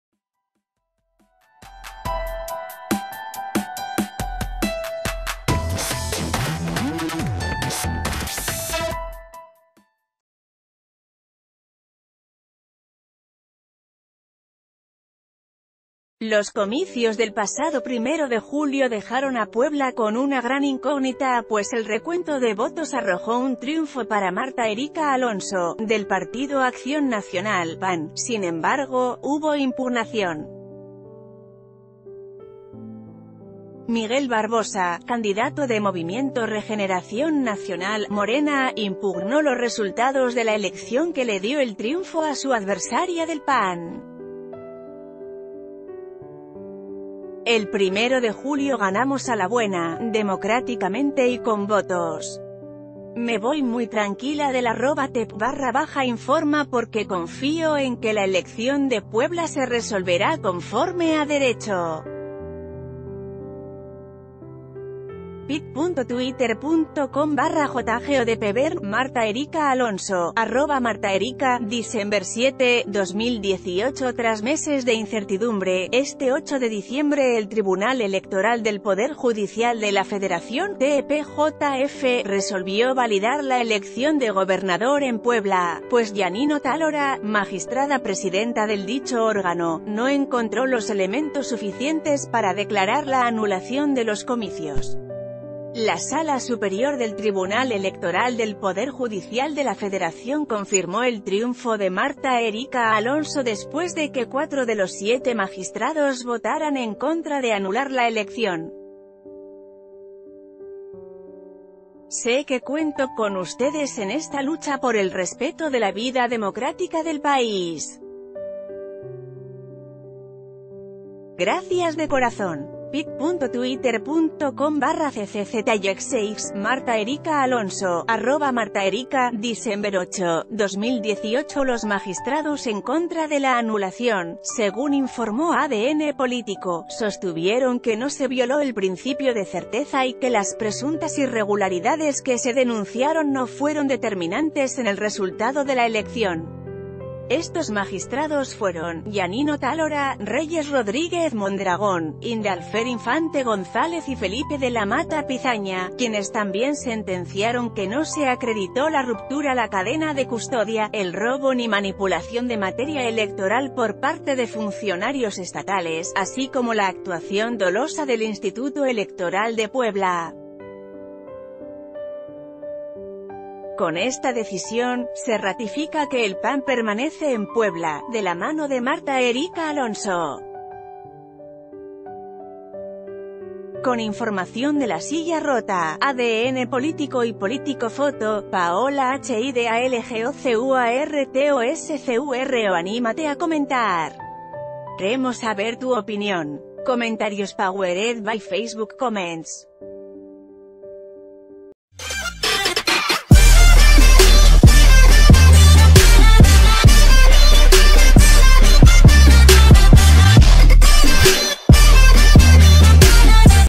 TEPJF ratifica elecciones en Puebla: Martha Erika Alonso se queda con la gubernatura
Los comicios del pasado 1 de julio dejaron a Puebla con una gran incógnita, pues el recuento de votos arrojó un triunfo para Martha Erika Alonso, del Partido Acción Nacional (PAN); sin embargo, hubo impugnación. Miguel Barbosa, candidato de Movimiento Regeneración Nacional (Morena), impugnó los resu...